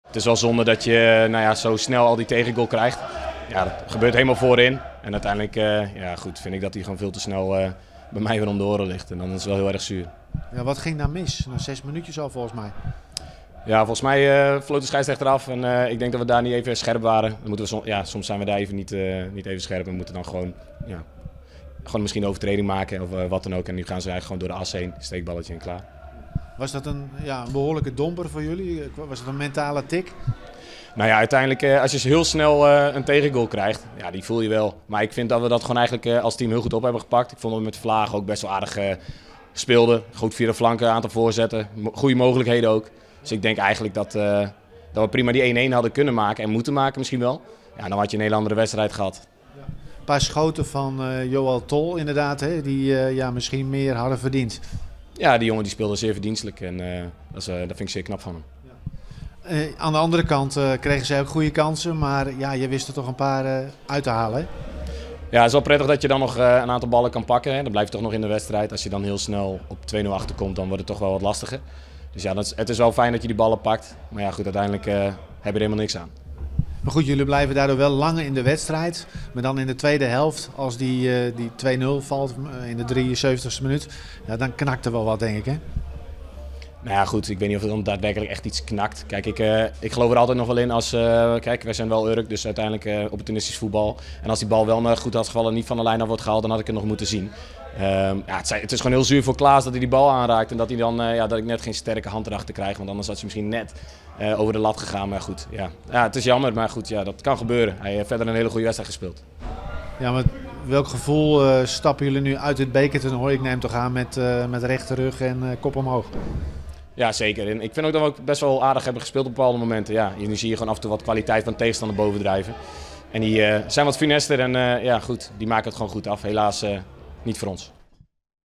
(2023-01-12) Nabeschouwing Urk-Katwijk met Richard Strijker
Nabeschouwing van de bekerwedstrijd tussen Urk en Katwijk, gespeeld op 12 januari 2023, met Richard Strijker.

Beelden: Omroep Flevoland